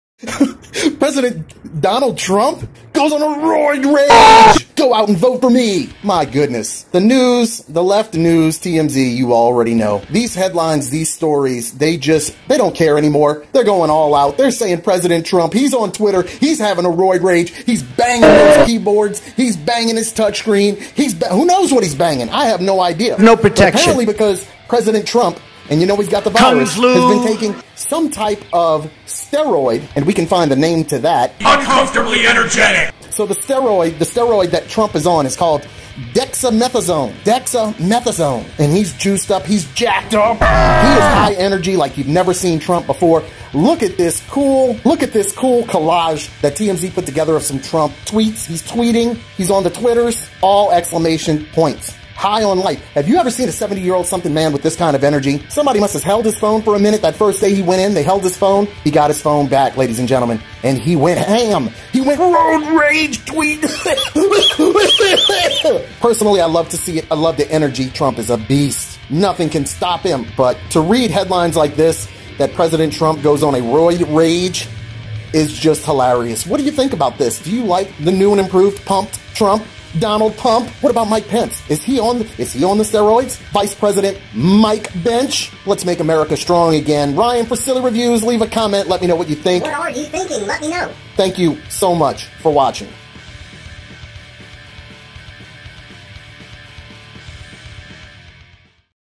Trump 'Roid Rage? Is Trump's use of Dexamethasone causing him to Tweet Uncontrolably?
does Trump have roid rage? President Donald Trump was prescribed dexamethasone to combat the symptoms of covid-19 coronavirus. after a day of virtually no tweets, President Trump has blasted the twittersphere with many tweets in all caps. Is it the steroids that are motivating this torrents of tweets? Is Donald Trump at super high energy levels now? could it be that Donald Trump simply has the caps lock button pressed on his keyboard? why else would Donald Trump be typing his tweets in all capital letters?